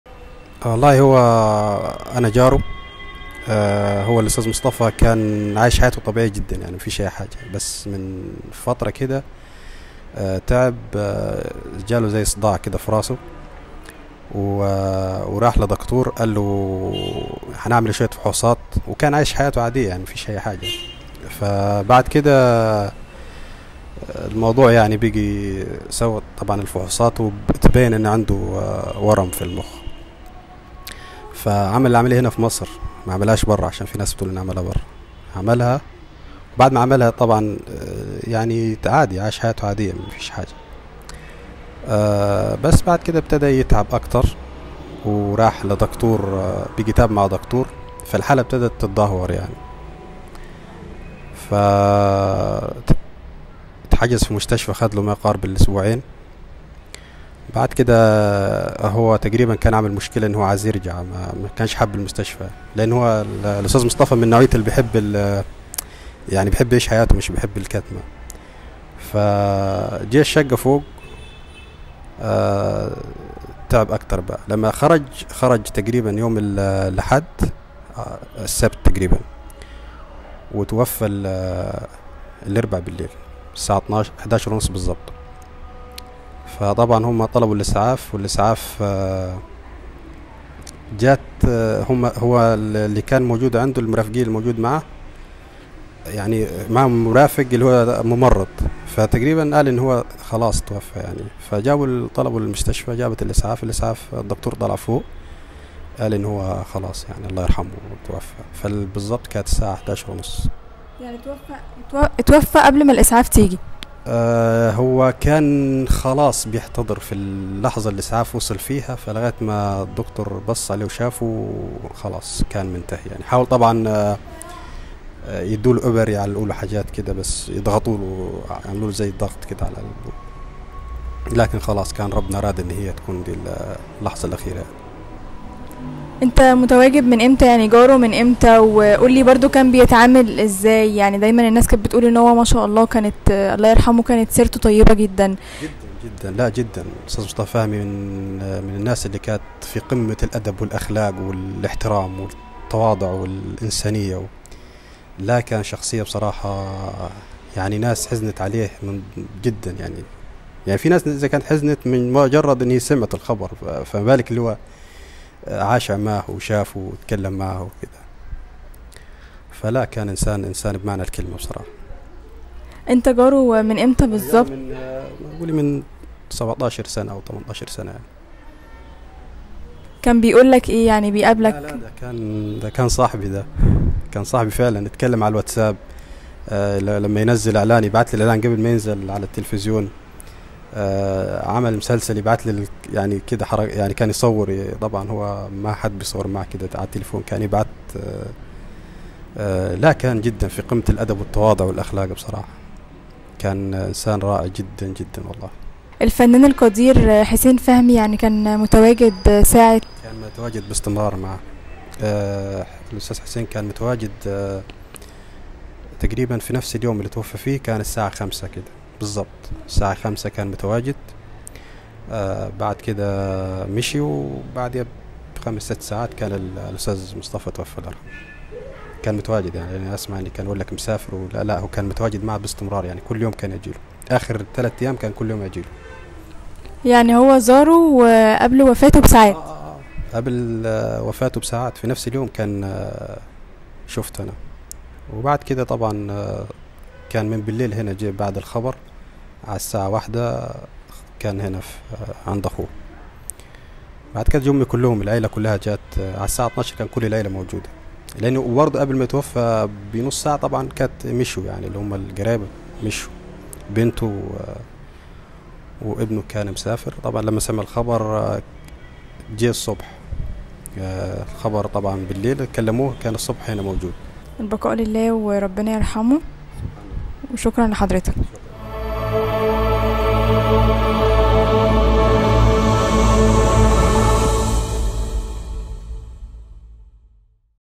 جار الفنان مصطفى فهمي يكشف اللحظات الأخيرة فى حياة دانجوان السينما المصرية
ـــــــــــــــــــــــــــــــــــــــــــــــ